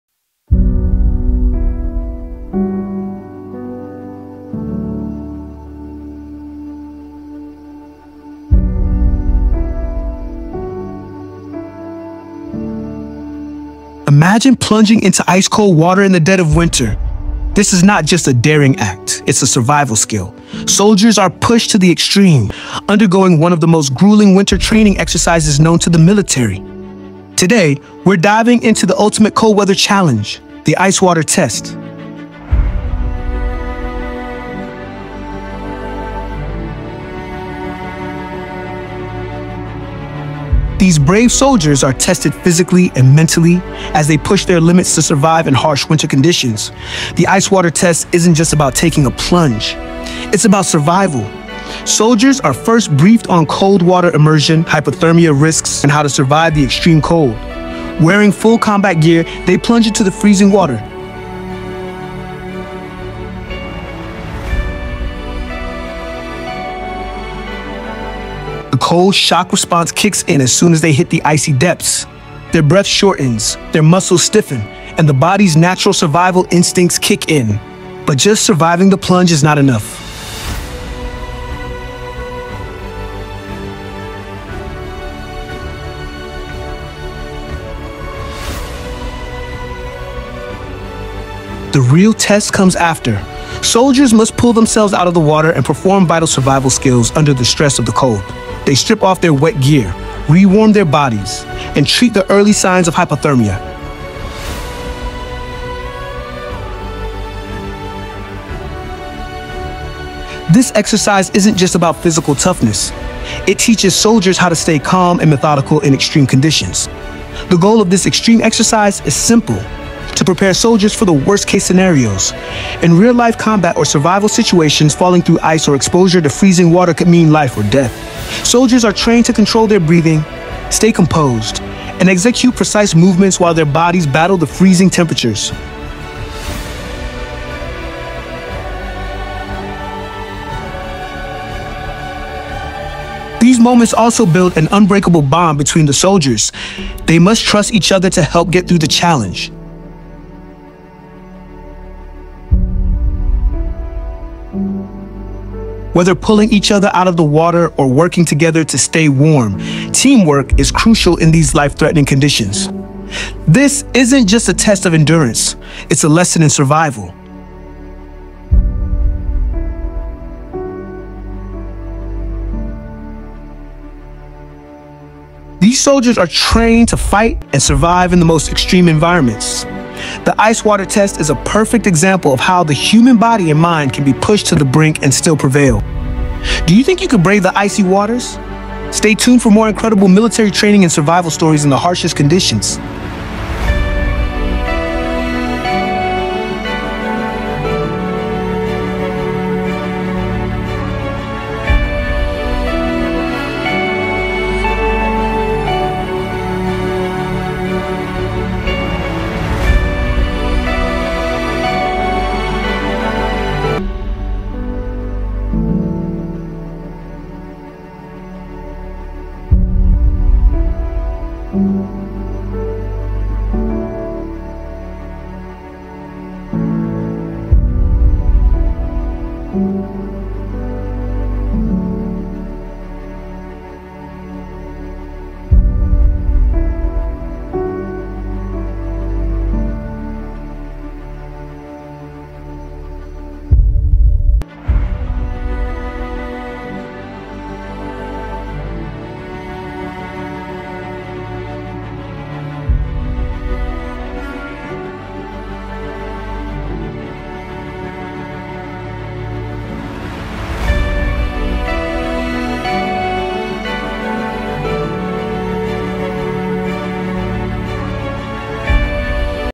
Brave Soldiers Take On the ICE BATH CHALLENGE of a Lifetime!
In one of the most intense challenges ever, brave soldiers take on the ultimate ice bath challenge! Watch as they push their physical and mental limits, submerging themselves in freezing cold water in an epic test of endurance and strength. Will they be able to handle the extreme temperatures and emerge victorious?
Brave Soldiers Take On the ICE BATH CHALLENGE of a Lifetime! This challenge isn't just about surviving the cold—it's about resilience, discipline, and mental toughness. Dive in with us and witness this extraordinary test of willpower, as these soldiers face the ice bath challenge of a lifetime!